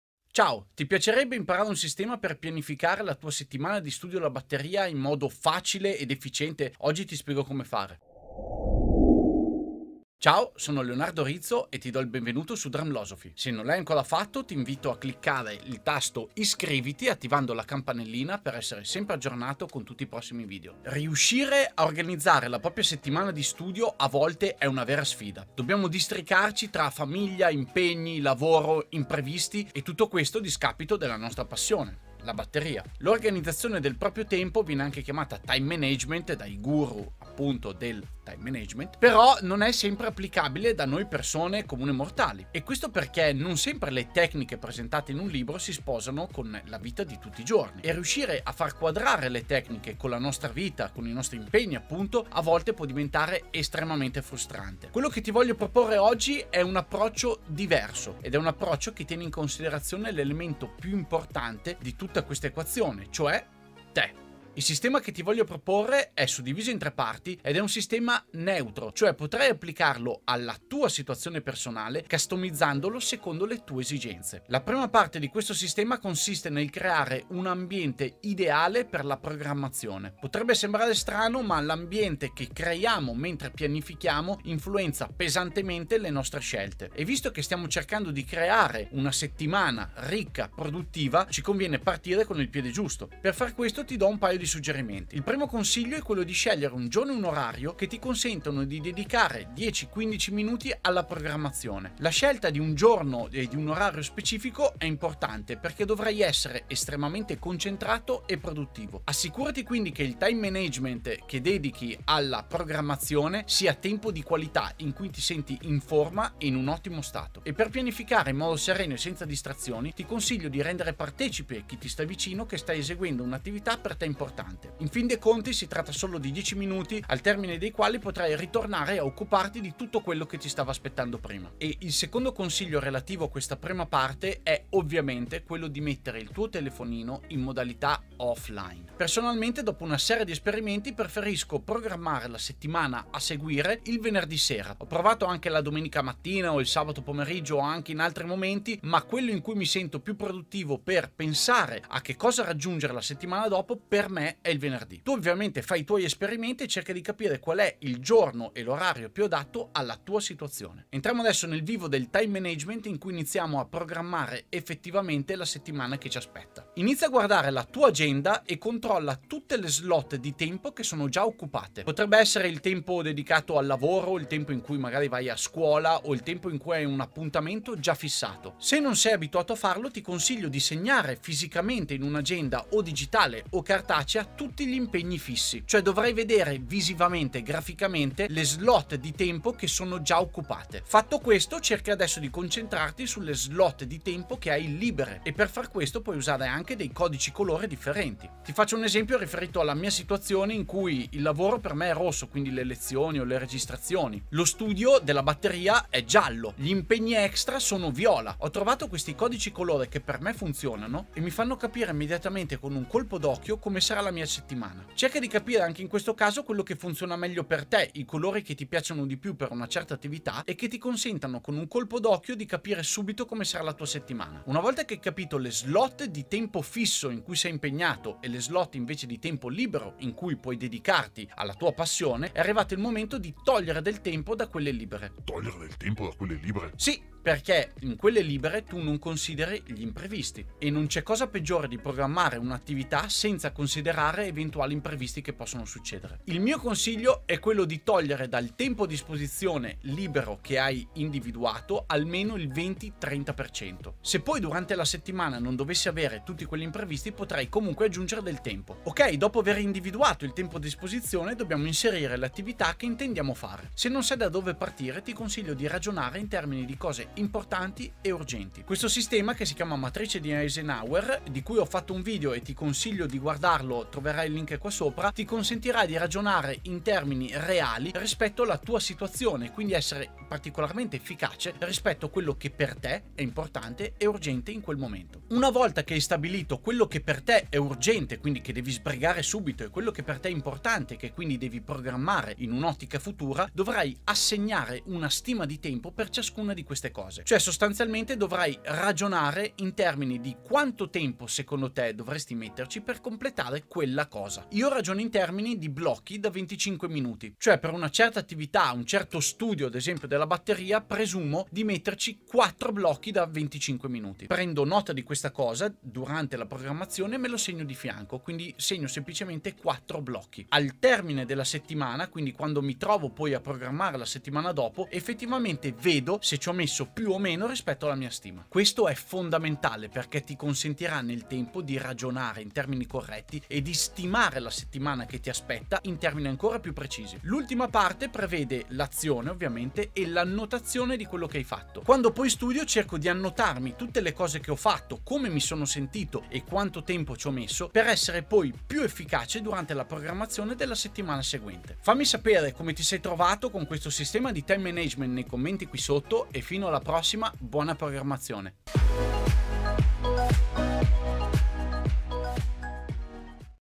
TIME MANAGEMENT alla batteria
Ciao, in questo video ti parlo di Time Management e pianificazione della settimana di studio alla batteria.
Il problema che negli anni ho riscontrato con i vari sistemi di pianificazione che ho studiato è che si adattavano poco e male alla vita di tutti i giorni! Ho deciso quindi di condensare le migliori strategie di Time Management e renderle fruibili in un sistema estremamente customizzabile secondo le tue esigenze e che ti permetta di avere il tempo che ti serve per studiare batteria.

STEP 1: CREA L'AMBIENTE IDEALE PER PIANIFICARE
La creazione di un ambiente stimolante e produttivo per pianficare la tua settimana di studio alla batteria impatta notevolmente sulla tua abilità di programmare efficacemente.
Ti consiglio di sperimentare e di trovare un giorno e un'ora che per TE e le tue esigenze siano ottimali per rimanere 10/15' concentrato sulla pianificazione. Rendi partecipi i tuoi famigliari o chi ti sta vicino che avrai bisogno di qualche minuto senza essere disturbato e che al termine sarai di nuovo disponibile per loro. E mi raccomando: Telefonino Off Line!

STEP 2: IDENTIFICA LE SLOT OCCUPATE E LIBERE
Inizia prendendo la tua agenda (digitale o cartacea) e identifica tutte le slot di tempo occupate da impegni fissi: Lavoro, appuntamenti, ecc
Utilizza dei codici colore per avere chiaro, al primo colpo d'occhio come è organizzata la tua settimana.
Passa poi a identificare le slot di tempo libere e, una volta identificate, togli il 20-30% dal tempo complessivo. Perchè? Beh perchè nella vita ci sono degli imprevisti che ci possono impedire di dedicarci allo studio della batteria. Perchè non imparare a prevederli?!

STEP 3: INSERIMENTO DELLE ATTIVITA'
Una volta che avrai chiaro il tempo a disposizione è venuto il momento di inserire le attività legate alla batteria nelle slot libere.
Se non sai da dove partire, prova a ragionare in termini di IMPORTANZA e URGENZA. Ho fatto un video su questo sistema che si chiama Matrice di Eisenhower. Inoltre inserisci anche una stima del tempo che pensi di metterci per il completamento delle attività che hai selezionato. Io ragiono in termini di blocchi da 25', quindi, di fianco a quello che devo fare quella settimana metto una stima basata su blocchi da 25'.
Questo aspetto è molto importante perchè, col tempo, ti permetterà di imparare a stimare correttamente quanto tempo ti ci vuole per il completamento di una certa attività.
Quando poi inizierà la settimana e ti metterai al lavoro, ricordati di annotare per ciascuno studio come è andata e quanto tempo ci hai messo. Questo parametro sarà da confrontare alla fine della settimana con la tua stima iniziale e ti servirà come verifica.

Fammi sapere cosa ne pensi di questo sistema di Time Management applicato allo studio della batteria.
Buona pianificazione!

Riceverai gratuitamente un planner pdf per organizzare le tue sessioni di studio

- Metti un like alla mia pagina FB ed entra anche tu nella community di DRUMLOSOPHY

- Puoi seguirmi anche su INSTAGRAM, TIK TOK e TWITTER digitando Drumlosophy